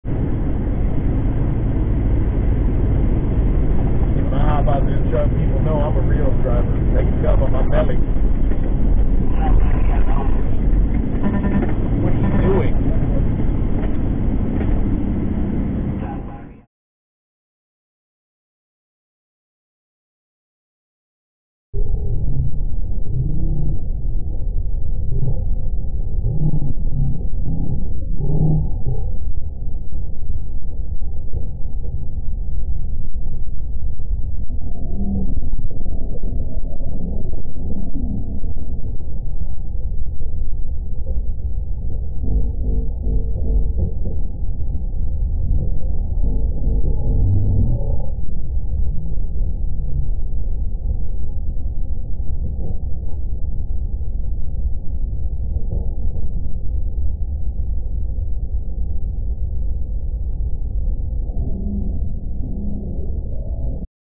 Mesilla Valley trucking. “Near miss”
Truck driving can be a dangerous job and safety is key.  Being alert and making split second decisions is critical. 65mph behind a car when they suddenly hard break to almost a stop to cut into the cones. My truck is fully loaded and very heavy. They probably don’t realize it but my driving saved their lives.